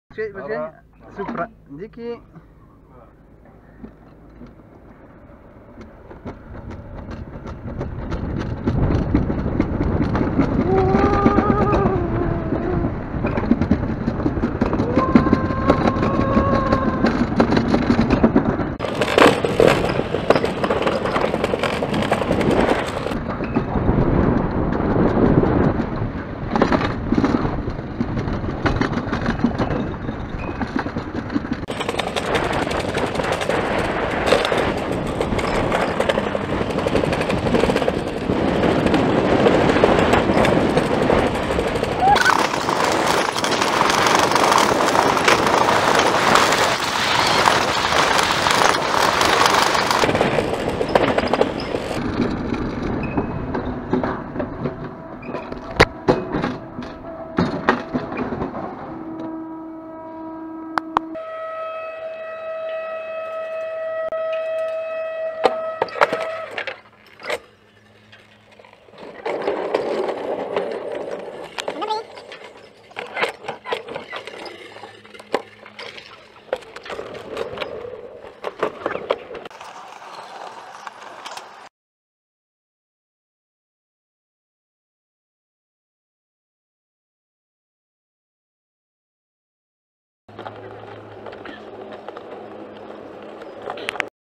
Menhirs on the road - Amazing bobsleigh in Poland
Travelling across Europe in Defender